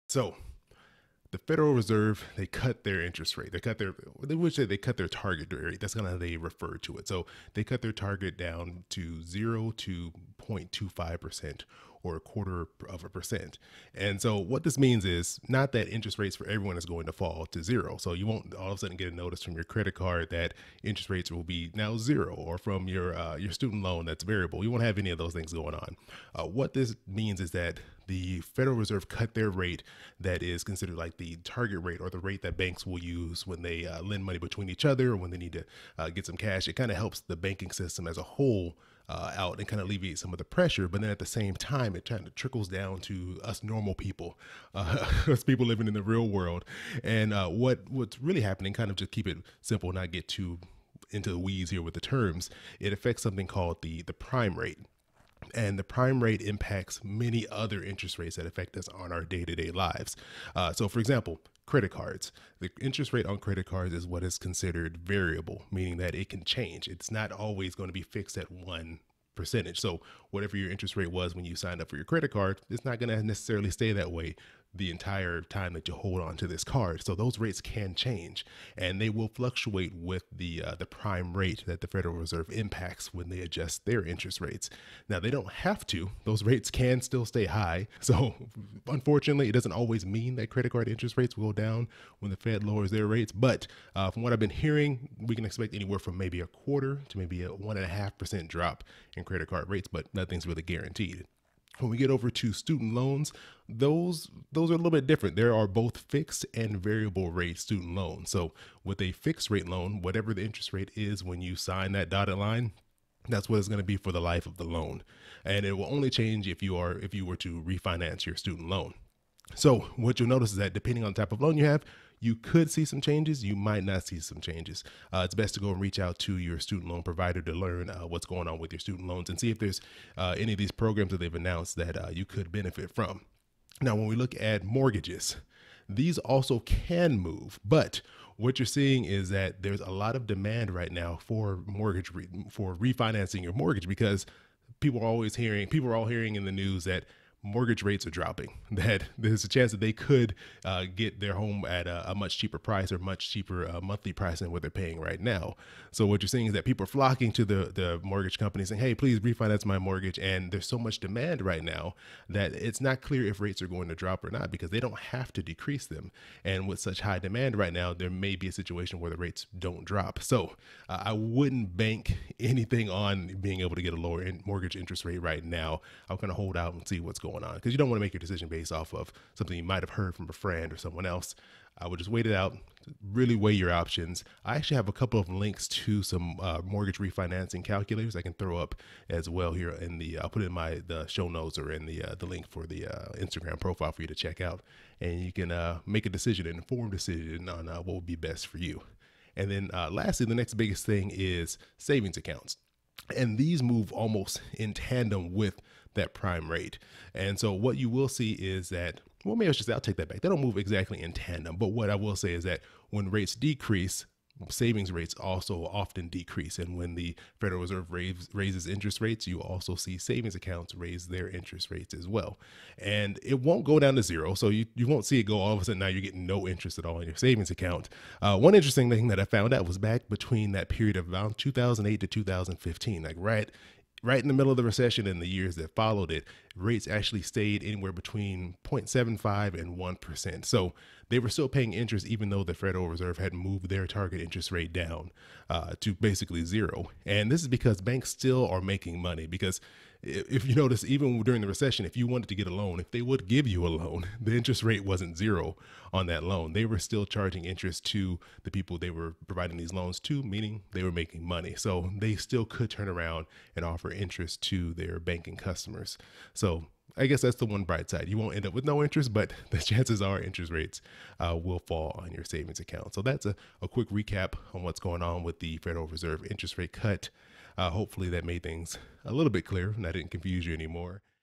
The Federal Reserve Cut Interest Rates
The federal reserve cut interest rates, what does this mean and how will it impact my finances?

I did an Instagram live earlier in the week but it went much longer than I expected. So I decided to record this quick video series to break down those explanations into smaller segments that you can enjoy in about the time it takes to make a bag of popcorn.